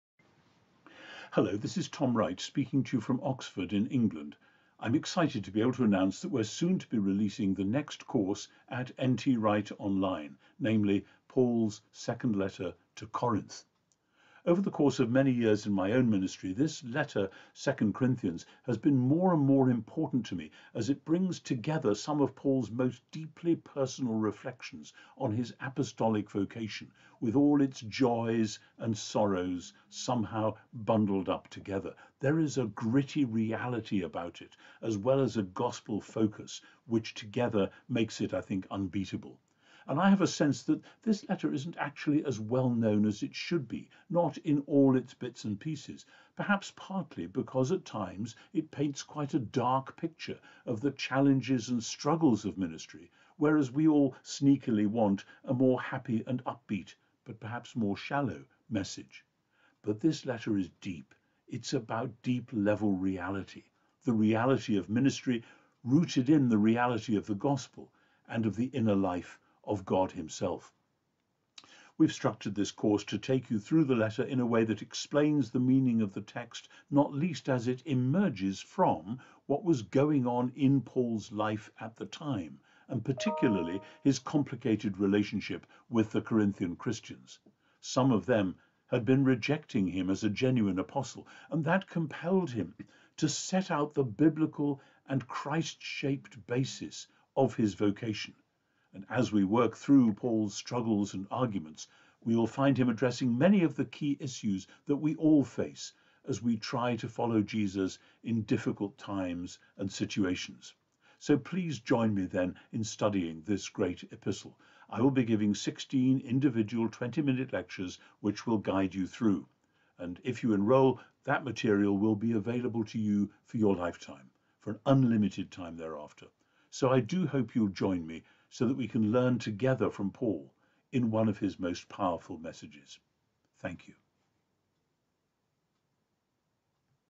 Paul’s Second Letter to Corinth | N.T. Wright Online
Guided by N.T. Wright, you can explore Paul’s theology of apostleship and ‘weakness’ and the significance for Christian life and ministry today.

The Apostle Paul writes a passionate letter that sheds light on the difficulties he experienced in his relationship with the Corinthian church. Prof. Wright teaches how Paul dealt pastorally with misunderstandings and assumptions in this church that he longed to correct. After his painful visit to Corinth, Paul writes from a distance and addresses the challenges to his apostleship and ministry with boldness and Godly sincerity. In this second letter to this fledging church, you will study how the practical issues of ministry and mission caused times of darkness for Paul. Yet, out of these struggles a fresh beam of light emerged in his life through his partnership with Titus, and ultimately from God who raises the dead. Through the ages, the Church has benefited from the Apostle Paul’s ‘theology of weakness’ and his vision of God’s strength and sufficiency through it all.

This is a timely and relevant course for followers of Jesus in today’s world to examine what is going on in terms of the larger Biblical story of God’s unbreakable love for his people. You will learn about God’s purposes to establish the Church as a Messiah-shaped family in which people live together with rebuke and forgiveness, grief and joy, and suffering and comfort. Prof. Wright will connect Paul’s vision of apostleship to Jesus’ death and resurrection, which will help you recognize when and how God is at work in your own life and the world.